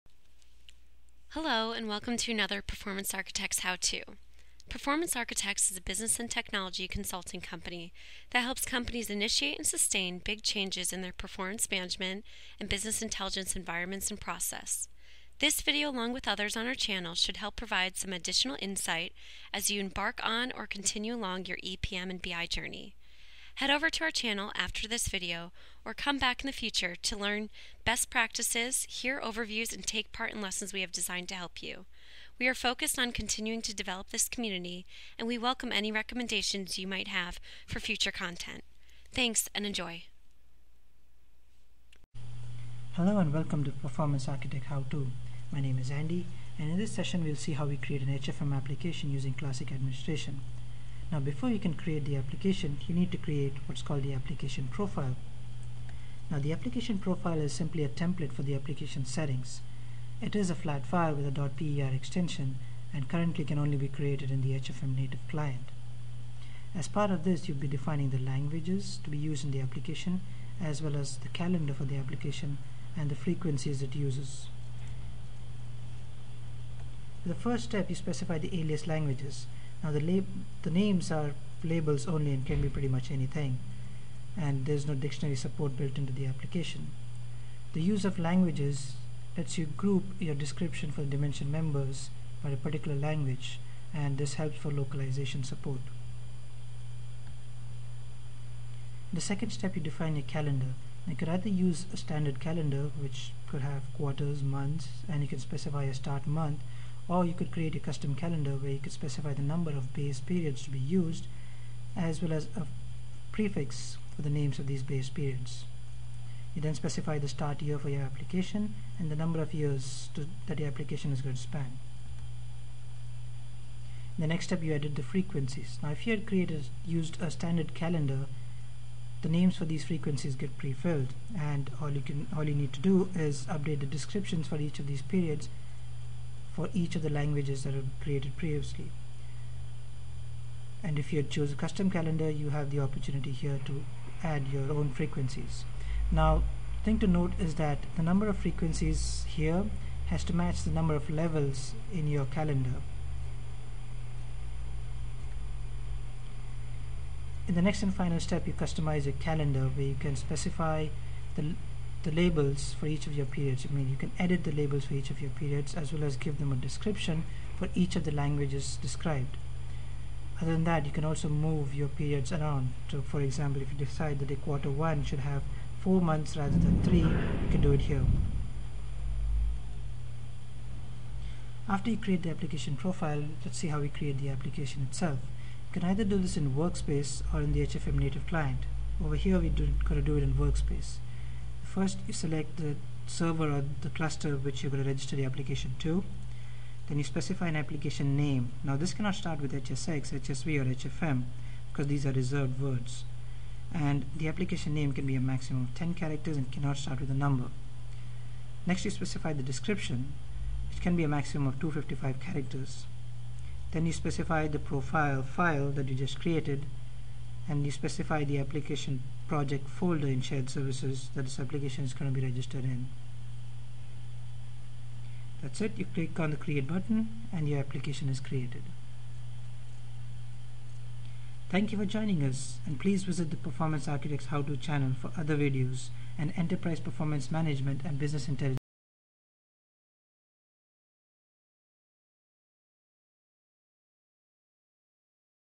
Creating an Oracle Hyperion Financial Management Application Using Classic Administration
This Performance Architects How To discusses how to create an Oracle Hyperion Financial Management (HFM) application using classic administration.